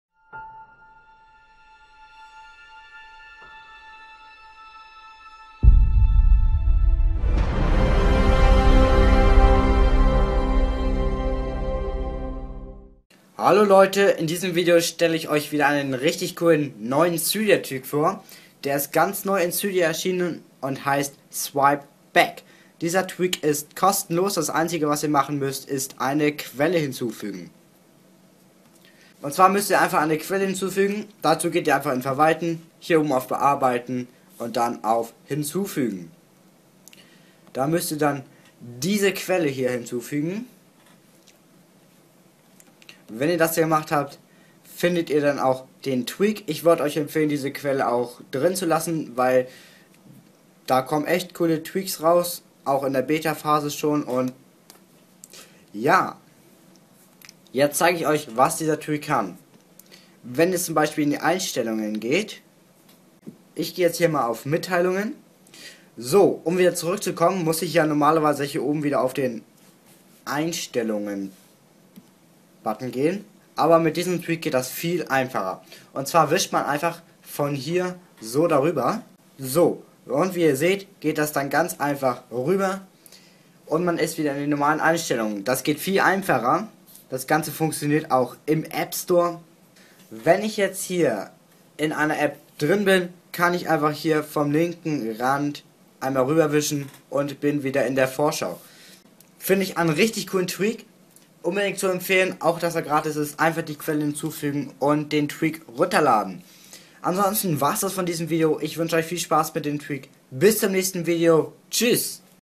SwipeBack - Sehr nützlicher Cydia Tweak
Hier stelle ich euch die kostenlose Cydia App "SwipeBack" vor. Damit könnt ihr sehr schnell wieder in die vorherige Ansicht einer App wechseln.
Tweak: SwipeBack
Preis: kostenlos